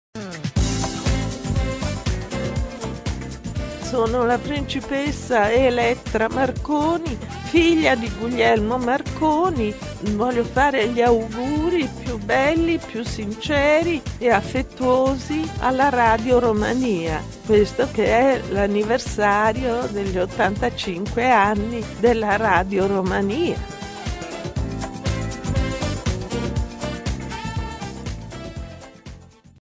Principesa Elettra Marconi pentru Radio Romania
"Sunt principesa Elettra Marconi, fiica lui Guglielmo Marconi, si doresc sa adresez postului Radio Romania urarile mele cele mai calde, sincere si afectuoase, pentru cea de-a 85-a aniversare pe care o sarbatoreste acum."